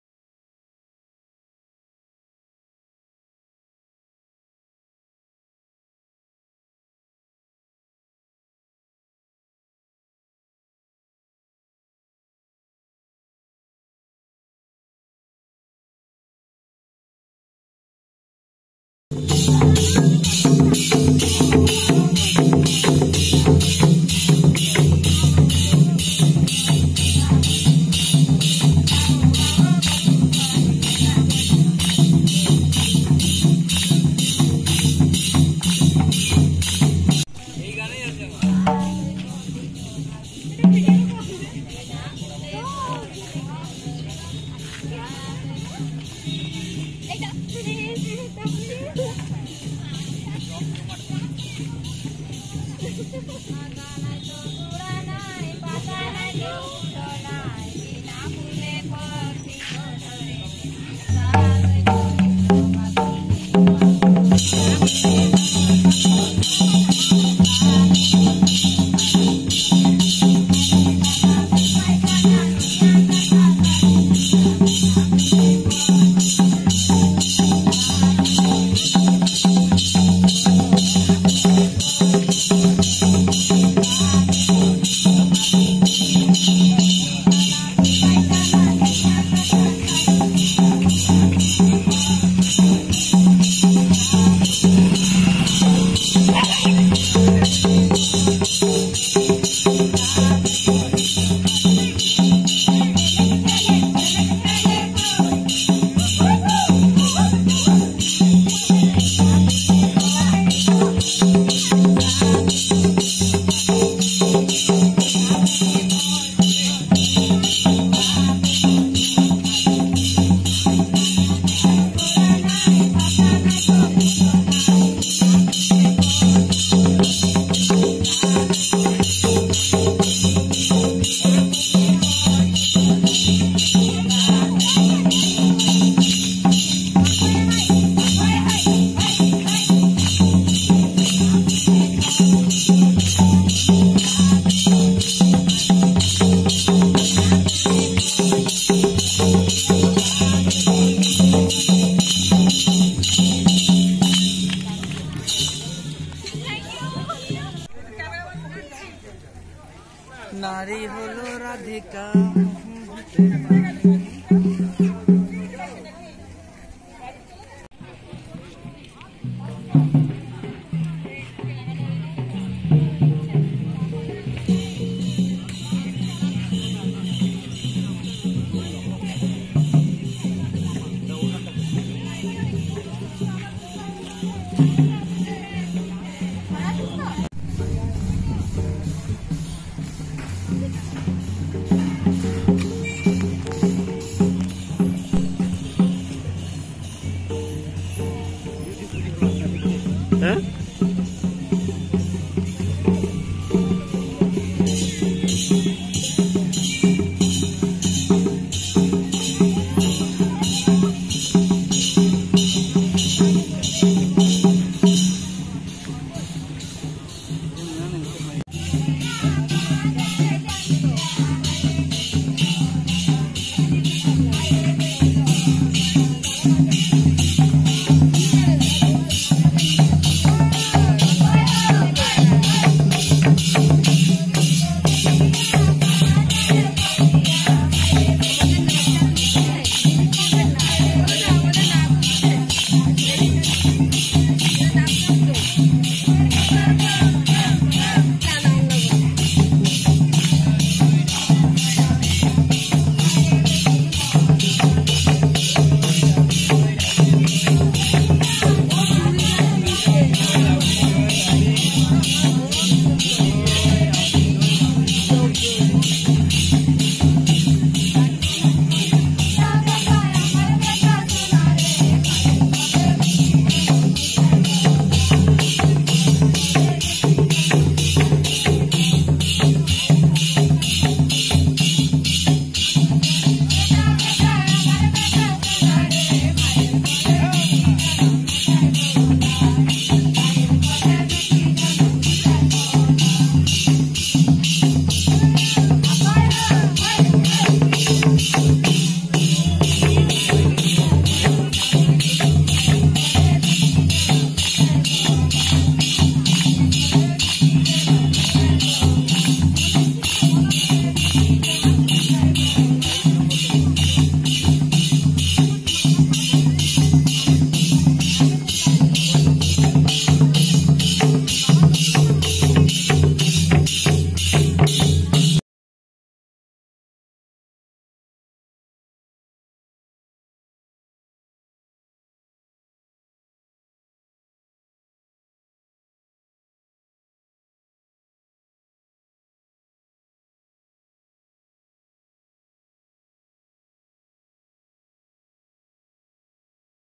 KHOAI MELA AT BOLPUR
SONAR JHURI,LOCATED AT BOLPUR,WEST BENGAL ,BIRBHUM DISTRICT.IT IS A TRADATION MELA.